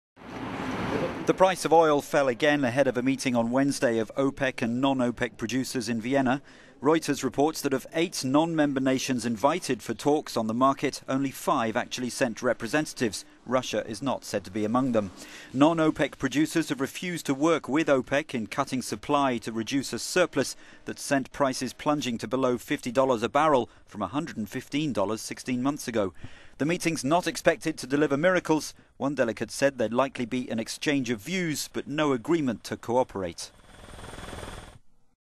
Oil producers meet amid little hope of agreement
The price of oil fell again ahead of a meeting on Wednesday of OPEC and non-OPEC producers in Vienna.

Reuters reports that of eight non-member nations invited to the talks on the market, only five actually sent representatives. Russia is not said to be among them.

Non-OPEC producers have refused to work with OPEC in cutting supply to reduce a surplus that has sent prices plunging to below $50 (44 euros) a barrel from $115 (101 euros) 16 months ago.